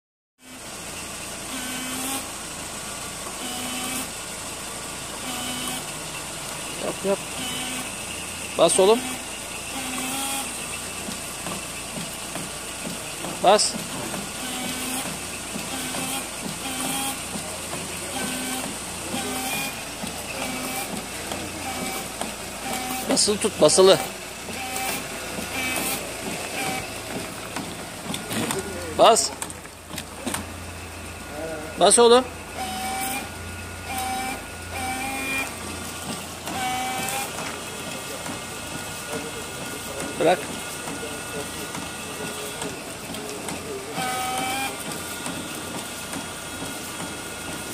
TİPO FAR YIKAMA ROBOTLARI | 🇹🇷TİPO TEMPRA 41🇹🇷 | Necmi ÜK
Tipo far yıkama robotları 🇹🇷TEMPRA TİPO 41 🇹🇷  Facebook grubu yöneticisi Necmi ÜK   ün aracı tipo 1.6 ie